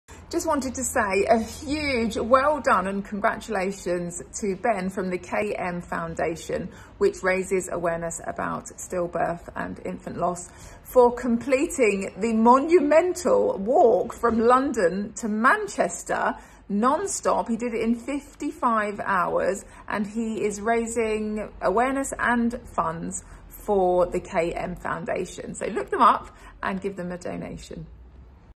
Jasmine Harman - A Place in the Sun
Ben received this wonderful message from the marvellous Jasmine Harman who you will know from A Place in the Sun.
Thank you Jasmine for your lovely words for Ben and our charity!